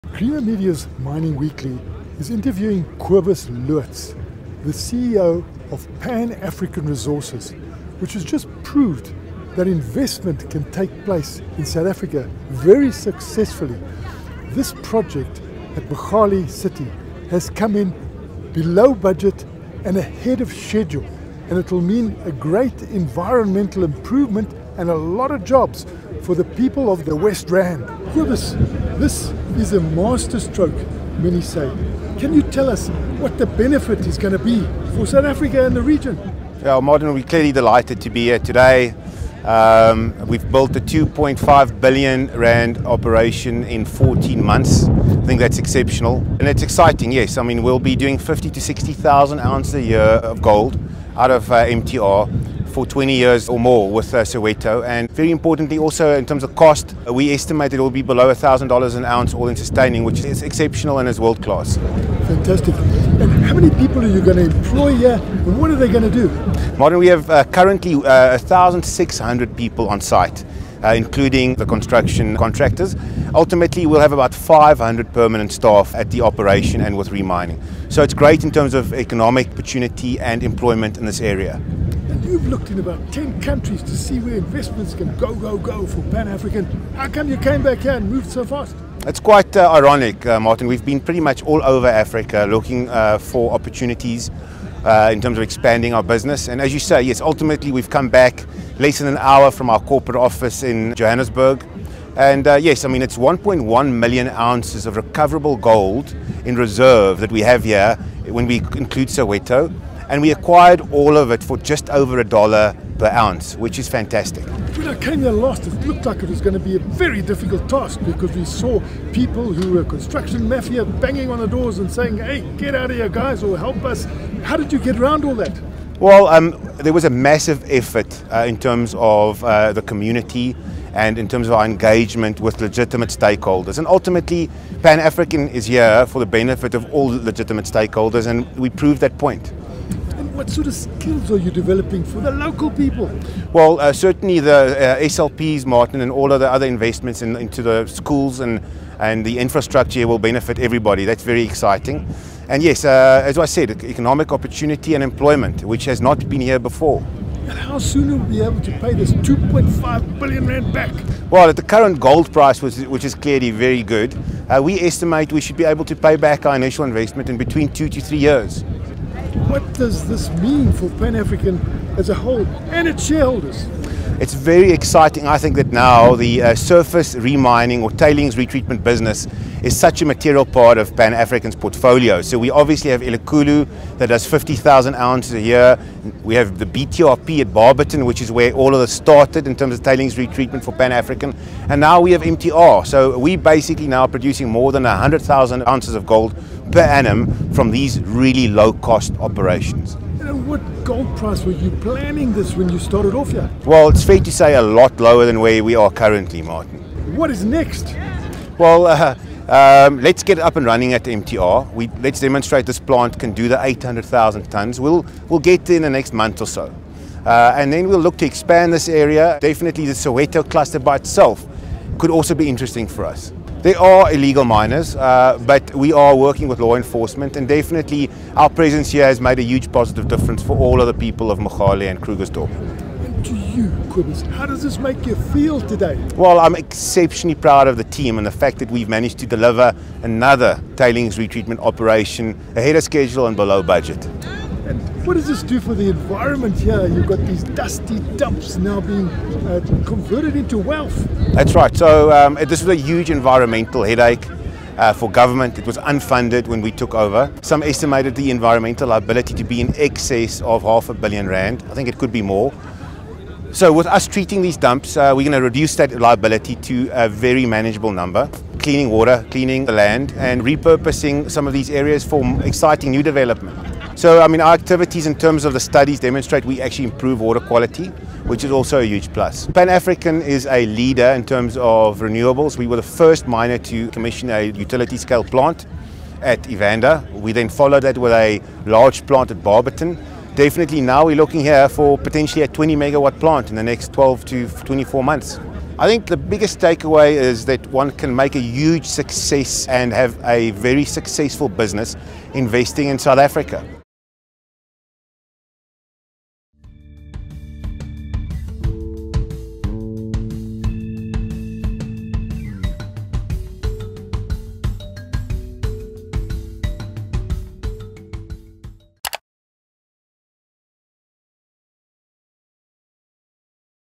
Pan African’s masterstroke gold operation gets going under budget, ahead of schedule
JOHANNESBURG (miningweekly.com) – The biggest takeaway from the hugely profitable Mogale tailings retreatment (MTR) operation, which was commissioned west of Johannesburg by Pan African Resources on Thursday, is that one can make a huge success and look forward to very rapid payback by investing in South Africa.